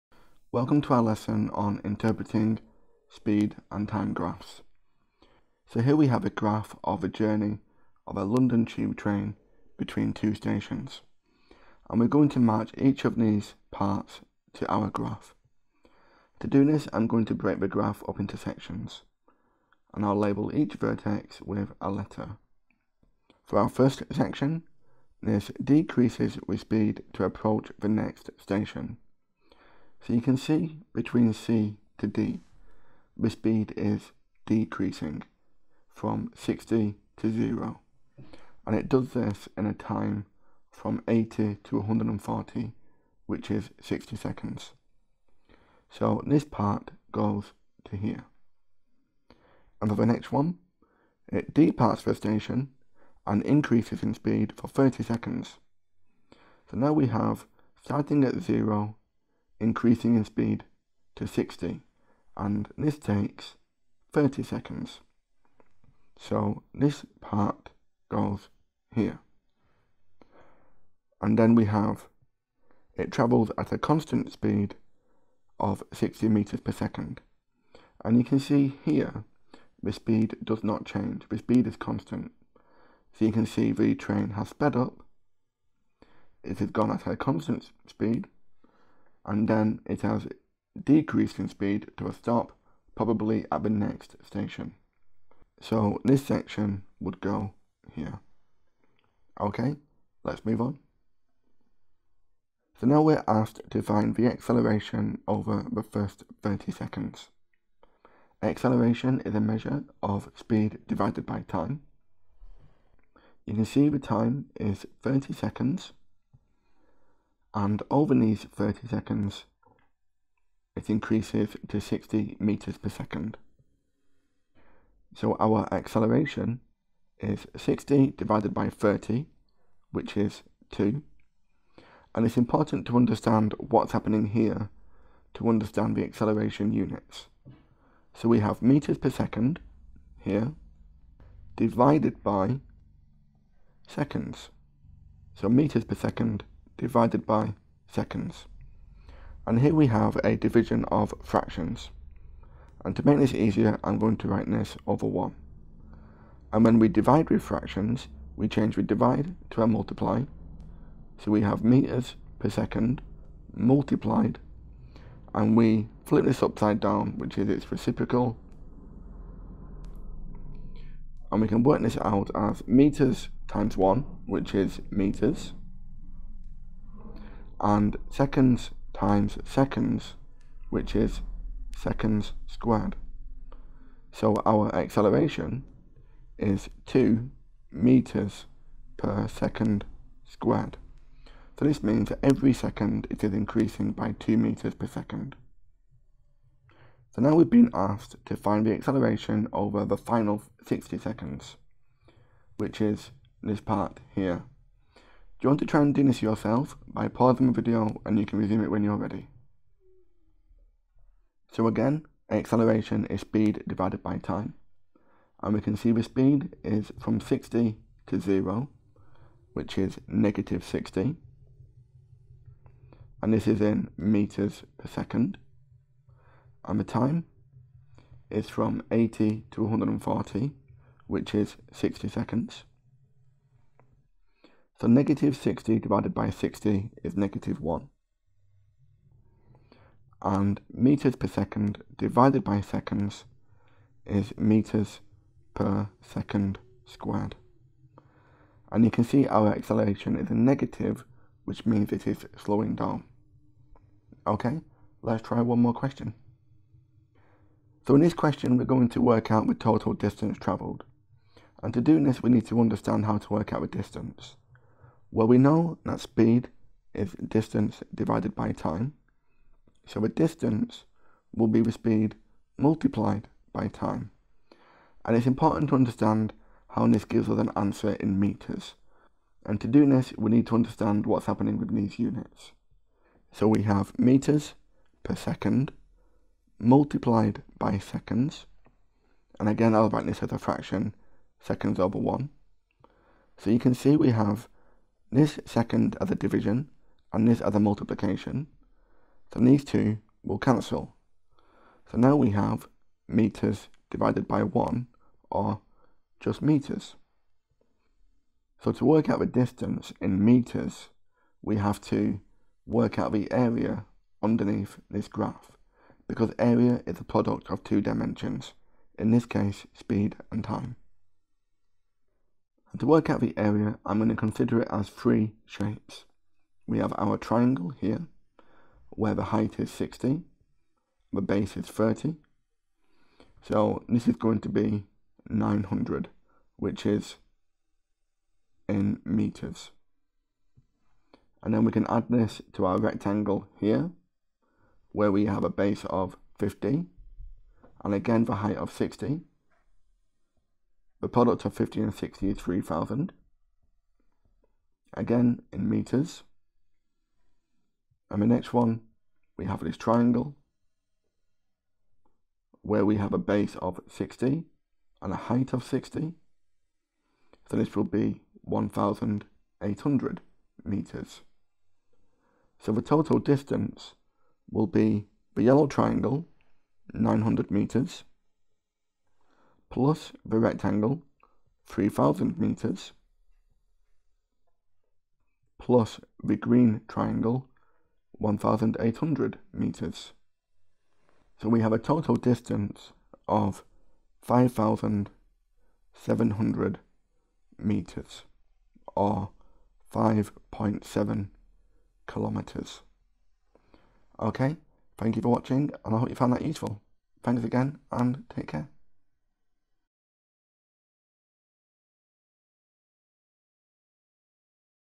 Speed vs time graphs - Mr Mathematics
How to  interpret speed vs time graphs to calculate acceleration and distance.